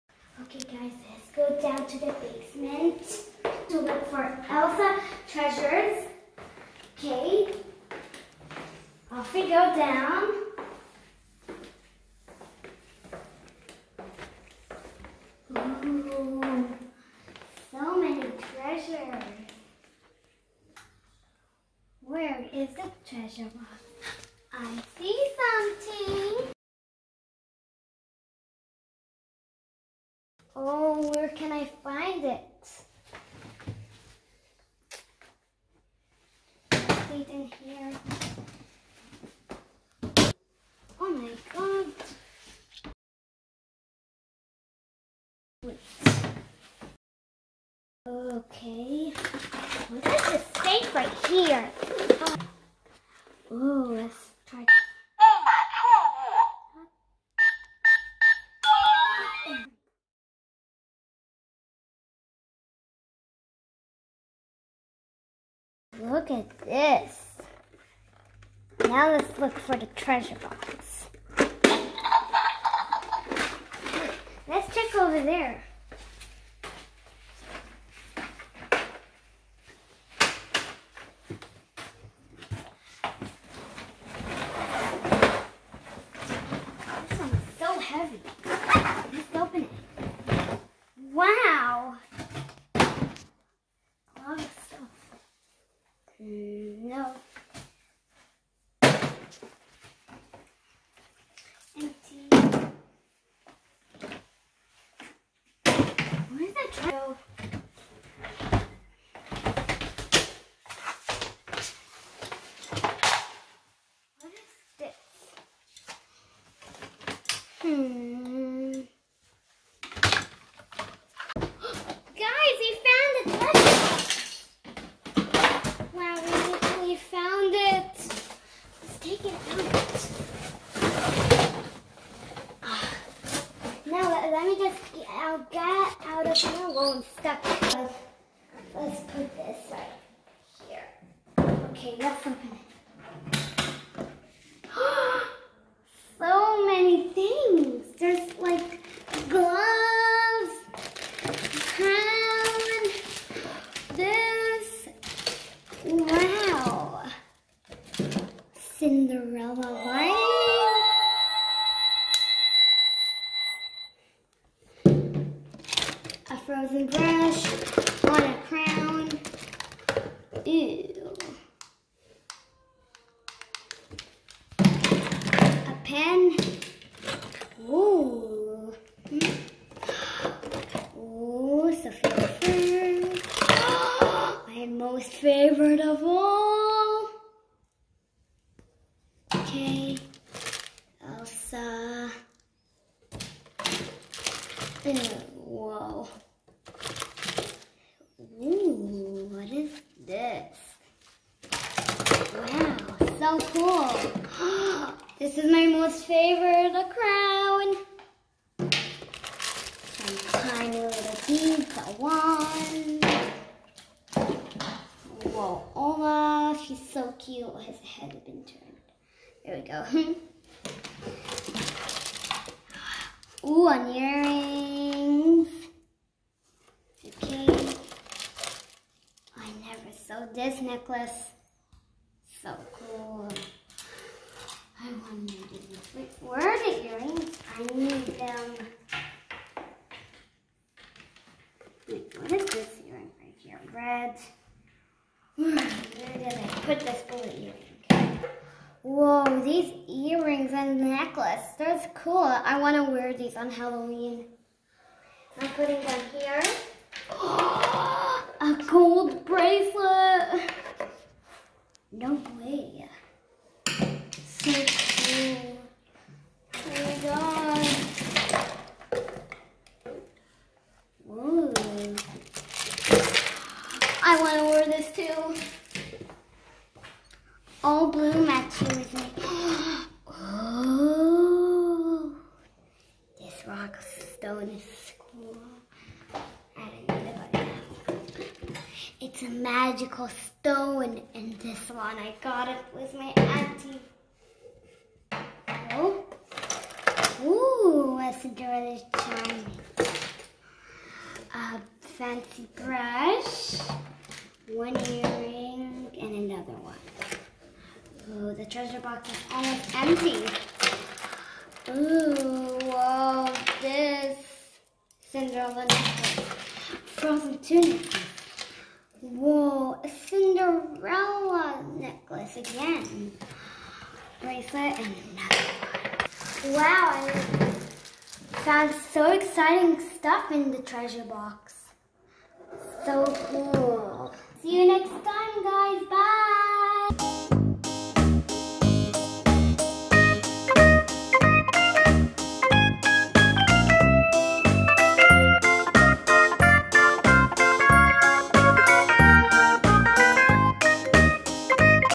Princess L | Elsa Looking for Her Magical Treasure Box Elsa's Treasure Hunt !!
Can Princess L find her treasure? Join Princess L while she searches for her treasure box!

Princess L’s World is a Learn and play channel for kids.